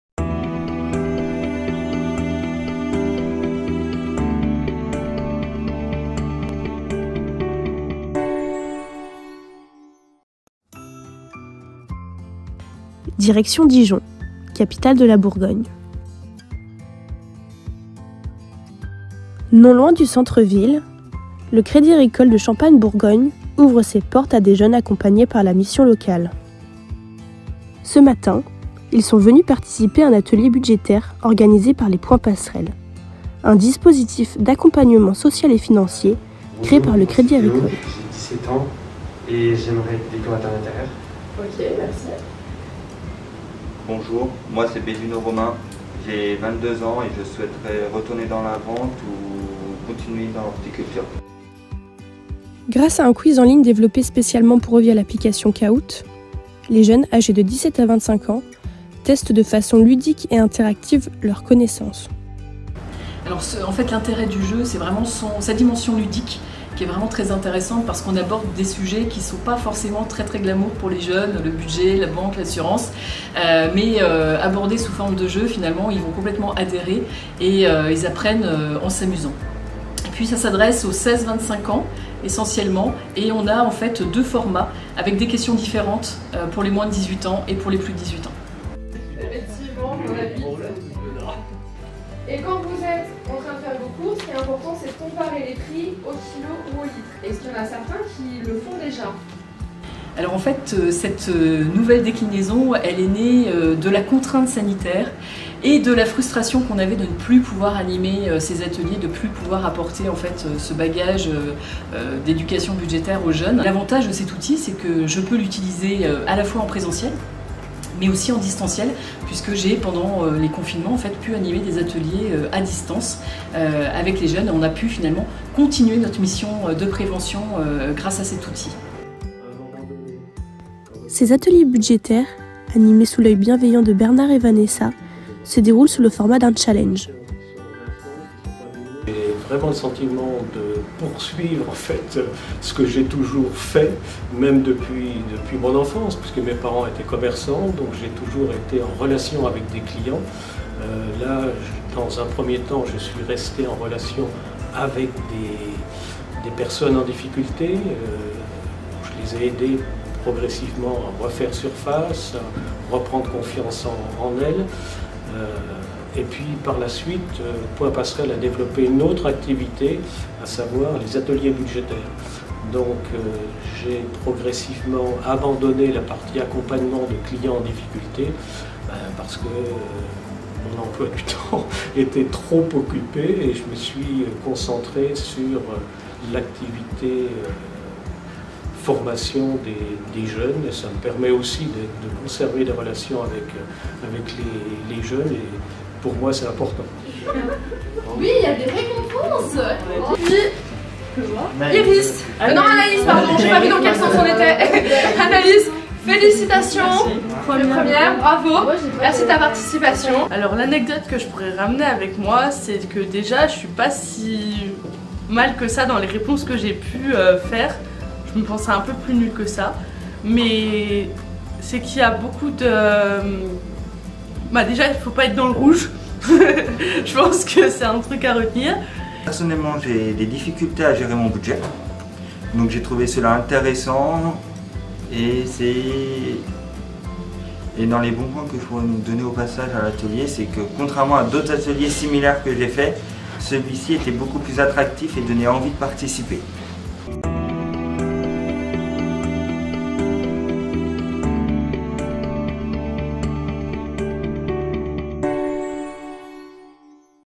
Les jeunes apprennent à gérer leur budget grâce aux Points Passerelle
A Dijon, le Crédit Agricole Champagne Bourgogne, ouvre ses portes aux jeunes de la Mission locale. Dans le cadre de leur projet en réinsertion, ils sont venus participer à un atelier budgétaire organisé par les Points Passerelles, un dispositif d’accompagnement social et financier créé par le Crédit Agricole.